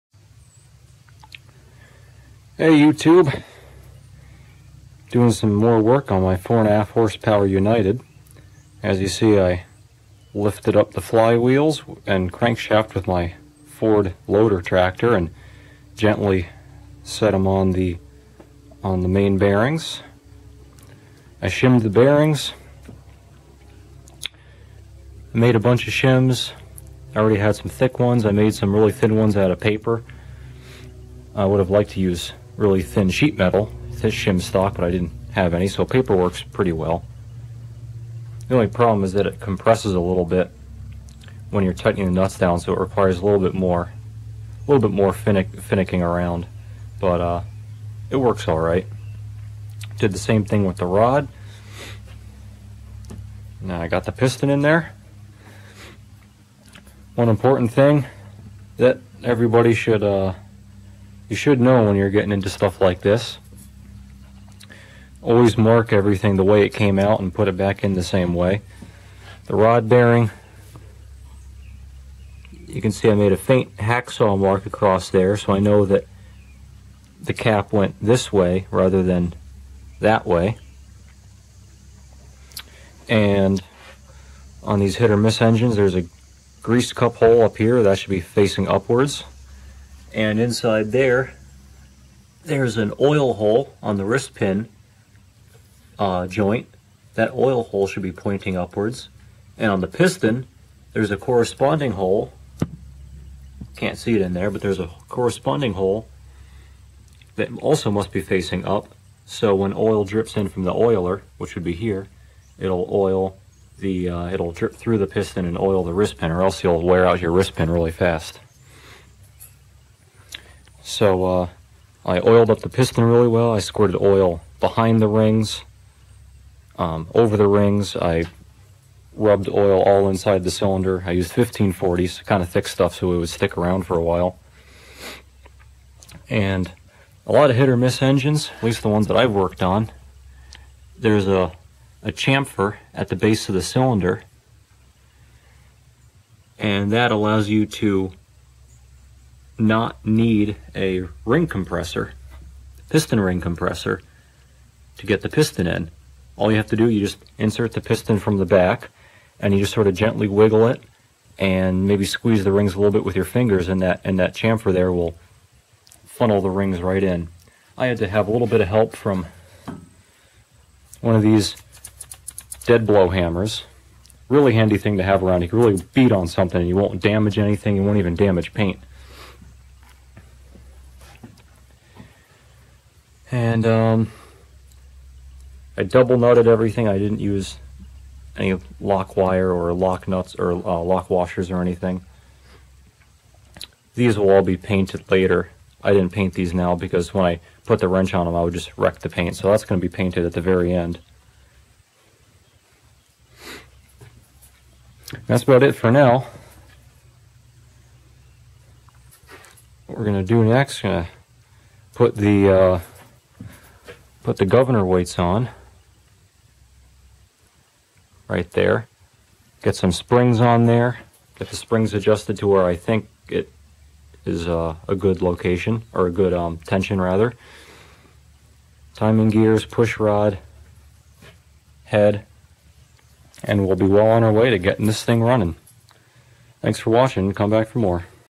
Working on the 4 1/2 HP United Hit and Miss engine - Shimming babbitt bearings and installing piston
We're in the home stretch!